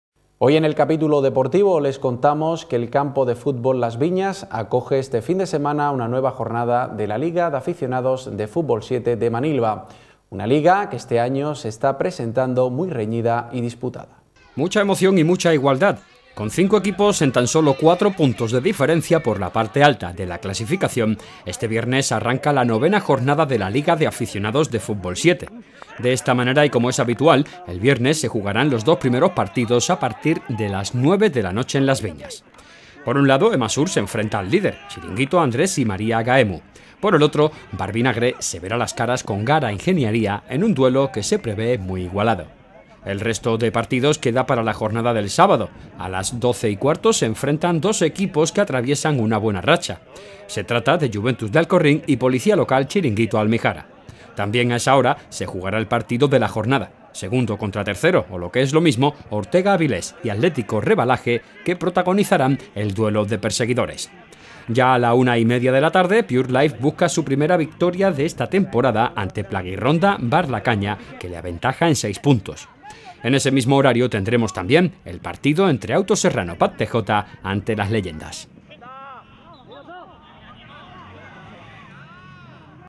NUEVA JORNADA DE LIGA DE AFICIONADOS DE FÚTBOL SIETE.
Mucha emoción y mucha igualdad. Con cinco equipos en tan solo cuatro puntos de diferencia por la parte alta de la clasificación, este viernes arranca la novena jornada de la Liga de aficionados de fútbol siete.

De esta manera y como es habitual el viernes se jugarán los dos primeros partidos a partir de las nueve de la noche en las viñas.

Por un lado, Emasur se enfrenta al líder, Chiringuito Andrés y María Gaemu. Por el otro, Bar Vinagre se verá las caras con Gara Ingeniería en un duelo que se prevé muy igualado.

El resto de partido queda para la jornada del sábado. A las doce y cuarto se enfrentan dos equipo que atraviesan una buena racha. Se trata de Juventus de Alcorrín y Policía Local Chiringuito Almijara.

También a esa hora se jugará el partido de la jornada. Segundo contra tercero o lo que es lo mismo, Ortega Avilés y Atl. Rebalaje que protagonizarán el duelo de perseguidores.

Ya a la una y media de la tarde Pure Life busca su primera victoria en esta temporada ante Plaguironda Bar La Caña, que le aventaja en seis puntos.

En ese mismo horario tendremos también el partido entre Autos Serrano Pub TJ ante Las Leyendas.